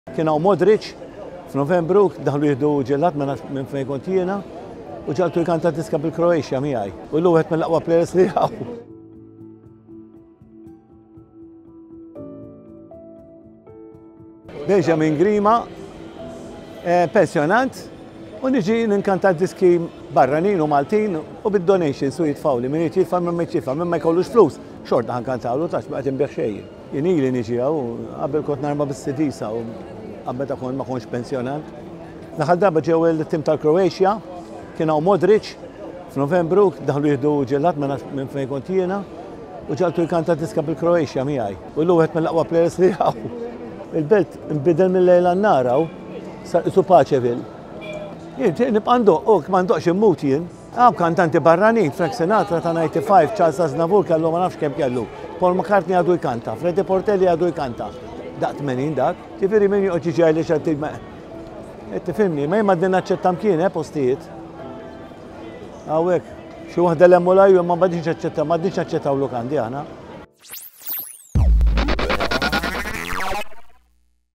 M Files: Benny Grima. 🎸
We met with local legend Benny, who you've definitely seen serenading the streets of Valletta if you've ever been to the capital. 🎤